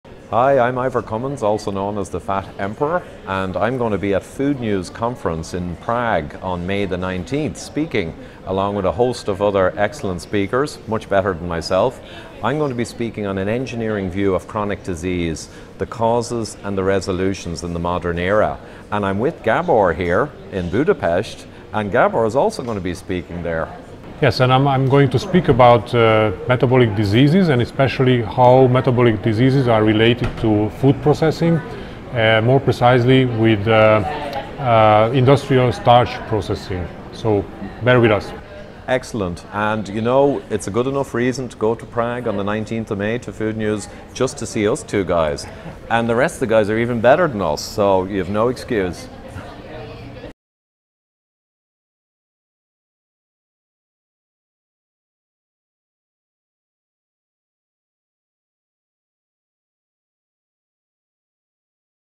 FoodNews Conference Prague - May 19th 2018
It's happening in Prague this May 19th - beautiful city, fantastic conference !  Interviewed the brilliant molecular biology expert Gabor Erdosi yesterday in Budapest - we covered food-processing impacts on obesity, cardiovascular disease and much more. The various interviews will soon be released, and you'll hear a lot more from FoodNews 2018 :-)